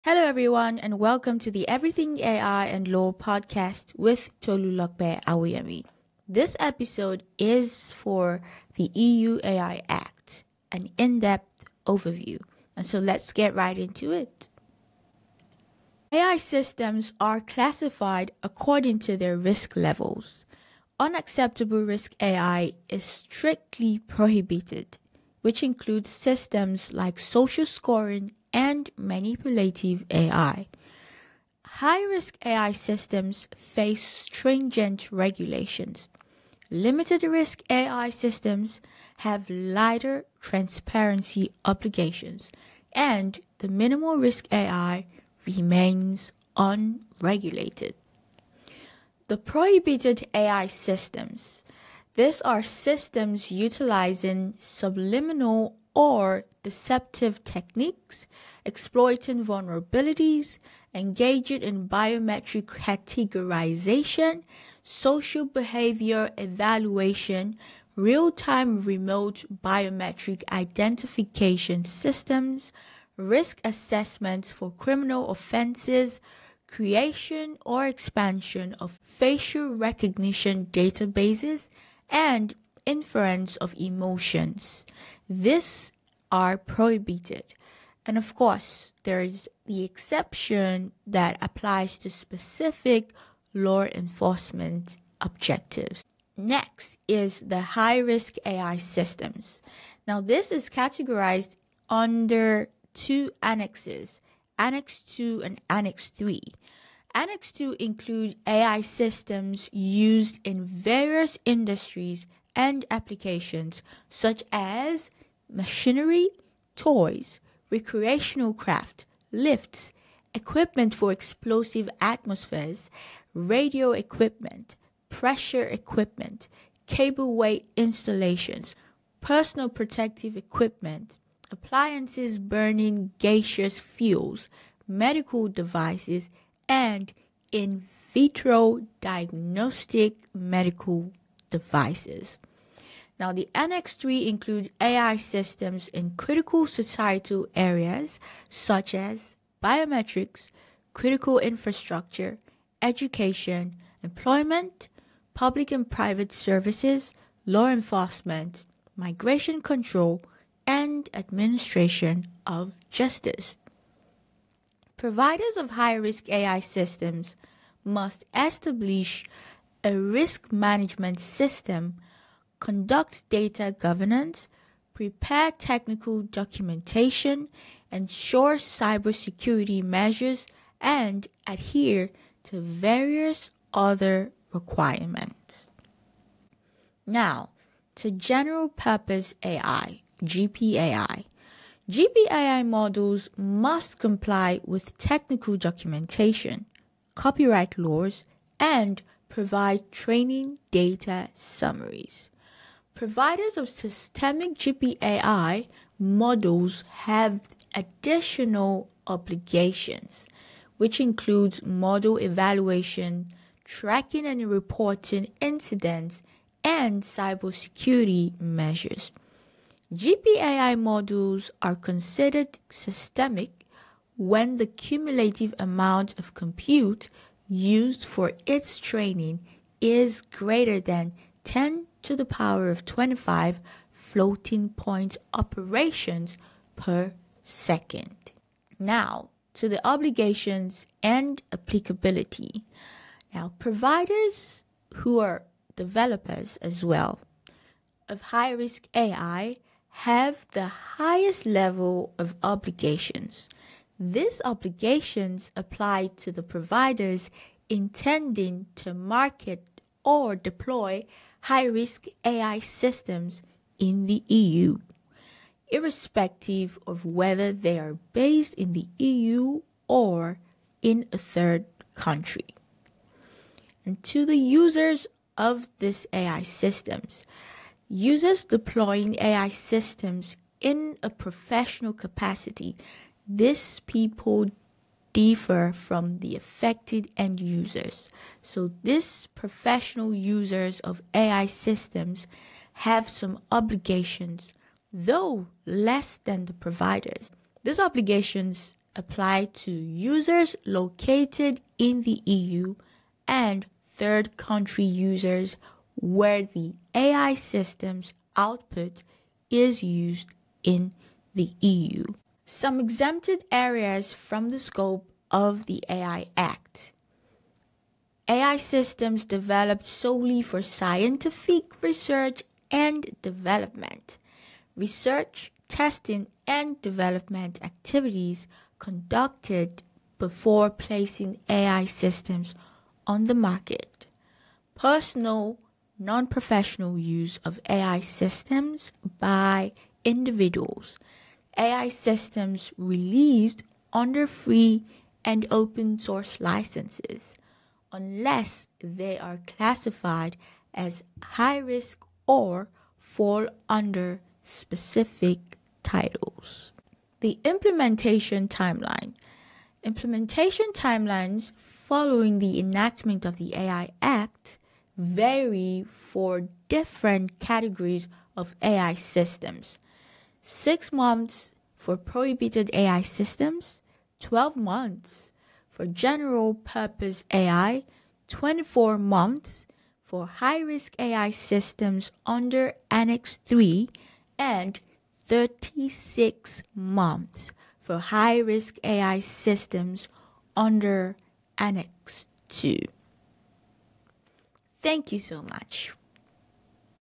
European Union (EU) AI Act Overview | AI Regulations Explained
The European Union AI Act is the first comprehensive legal framework for regulating artificial intelligence. In this episode, I break down the key components of the Act, including AI risk classifications, prohibited systems, obligations, and implementation timelines.

Key Topics Covered:
✅ Classification of AI by Risk Levels – How the EU regulates AI based on potential harm
✅ Prohibited AI Systems – AI technologies that are banned under the Act
✅ High-Risk AI Systems – AI applications with strict compliance requirements
✅ General Purpose AI (GPAI) – Understanding its regulatory impact
✅ Obligations & Applicability – Who must comply with the AI Act?
✅ Exempted Areas – AI systems not covered under the regulation
✅ Implementation Timeline – When will the EU AI Act take full effect?

This episode is essential for legal professionals, AI developers, business leaders, and anyone interested in AI governance and compliance.

Chapter Timestamps:
⏱ 00:00 - Introduction
⏱ 00:16 - Classification of AI by Risk Levels
⏱ 00:49 - Prohibited AI Systems
⏱ 01:36 - High-Risk AI Systems
⏱ 03:06 - General Purpose AI (GPAI)
⏱ 03:58 - Obligations & Applicability
⏱ 05:12 - Exempted Areas
⏱ 05:57 - Implementation Timeline

💬 How do you think the EU AI Act will impact global AI regulation? Let’s discuss in the comments!